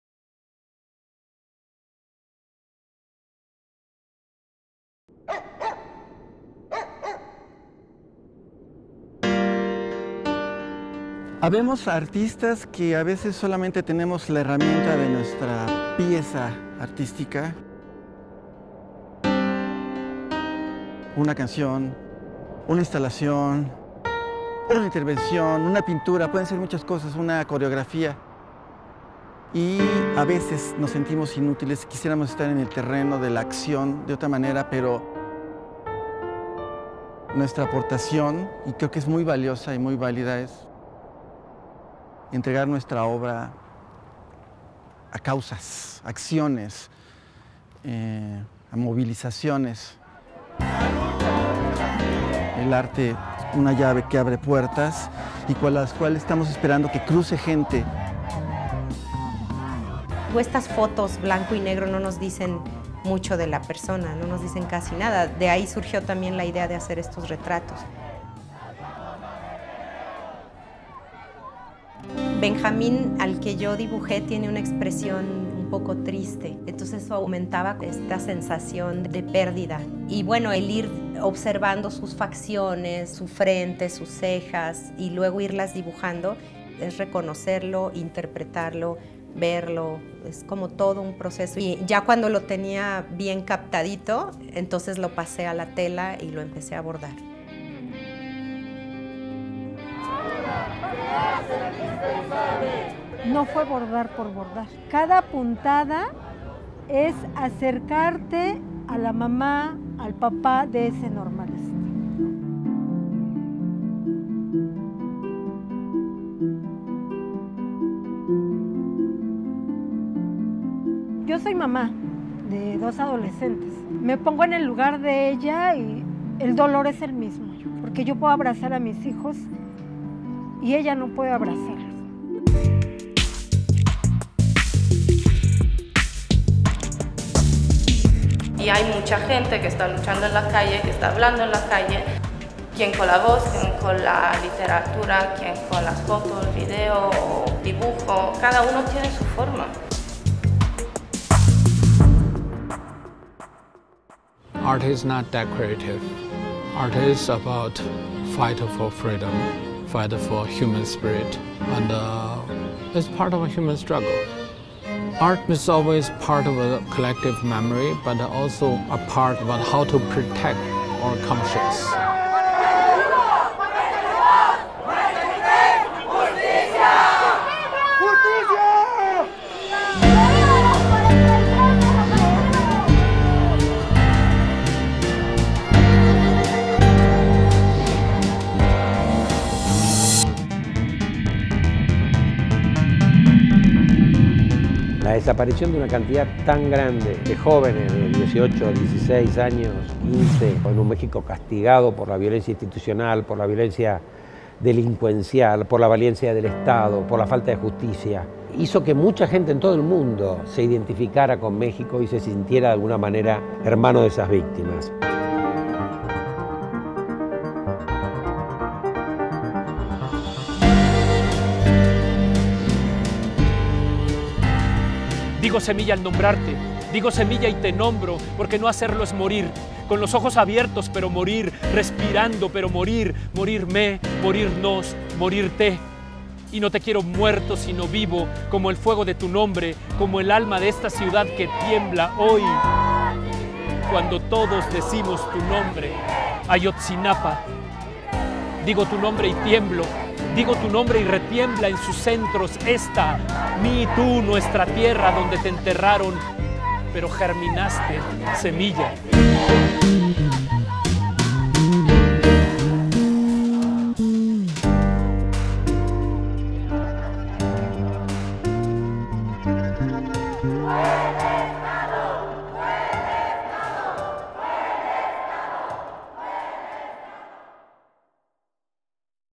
Este arte que abraza | Función de bienvenida y despedida 19°DocsMX
Este arte que abraza
México | 2024 | 116 min | español
Dirección Juanfe Castro Gessner
Evocación de las manifestaciones artísticas genuinas que fueron incitadas por el movimiento de los 43 desaparecidos de Ayotzinapa, la carga de pensamiento y profundísimo discurso que generó el caso en las artes.